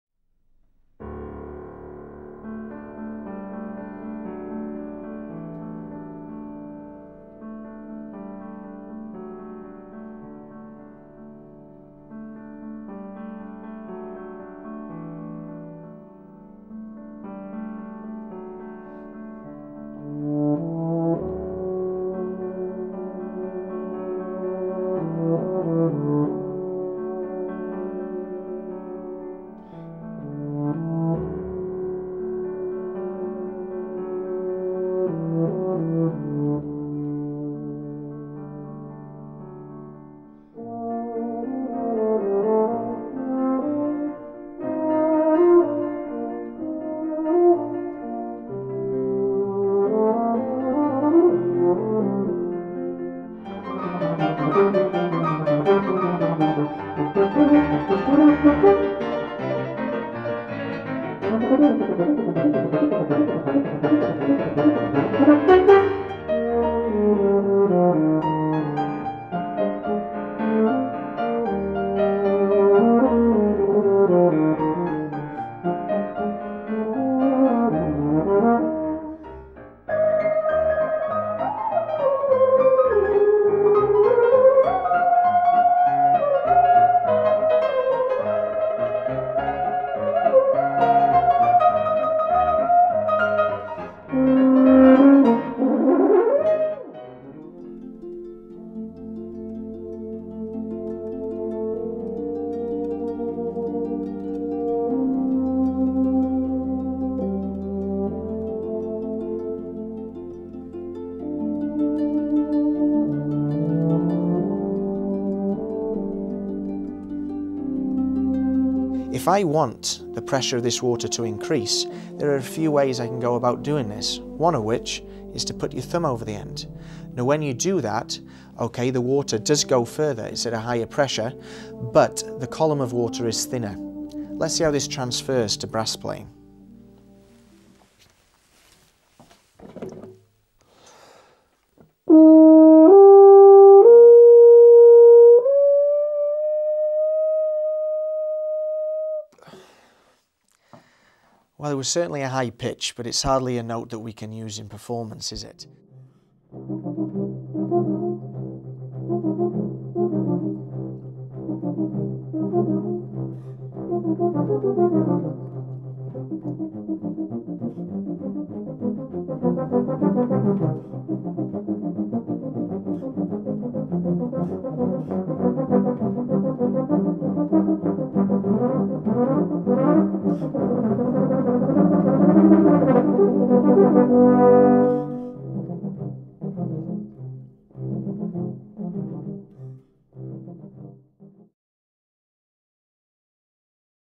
David Childs - A Master Class in Brass DVD trailer
Regarded as one of the finest brass soloists in the world today, David Childs is also a highly sought-after teacher, holding teaching posts at the Royal Welsh College of Music and Drama and Birmingham Conservatoire. On this new DVD, David comes into your home to bring you his personal thoughts on developing as a brass player. Not just for euphonium players, his help and advice applies to all brass instrumentalists. Also included are ten stunning solos performed by David himself.